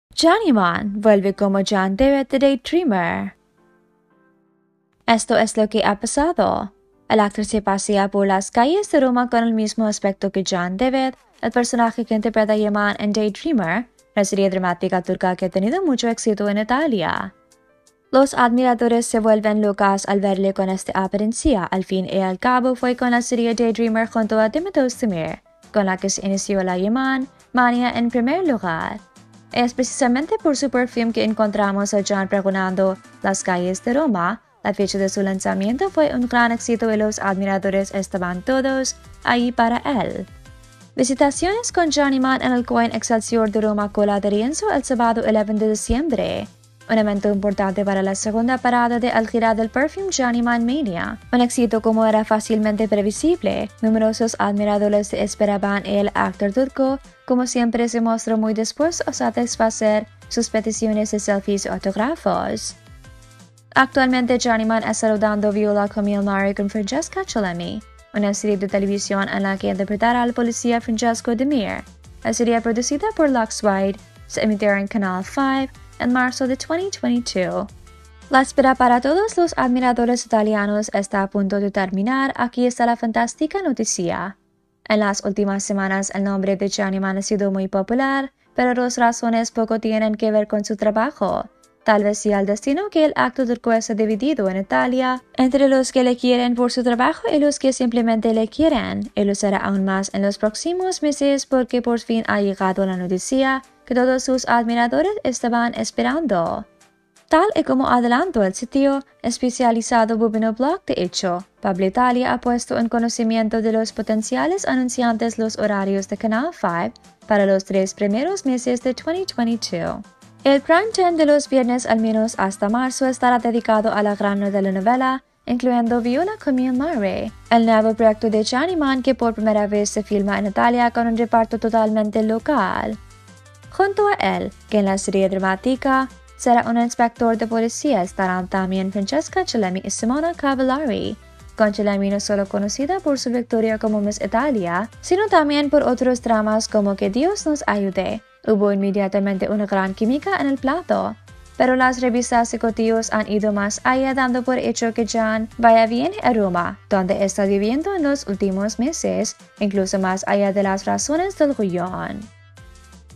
Can Yaman y Demet Özdemir: Vuelve como Can Devit de DayDreamer. Esto es lo que ha pasado
Can Yaman y Demet Özdemir: Vuelve como Can Devit de DayDreamer.
Esto es lo que ha pasado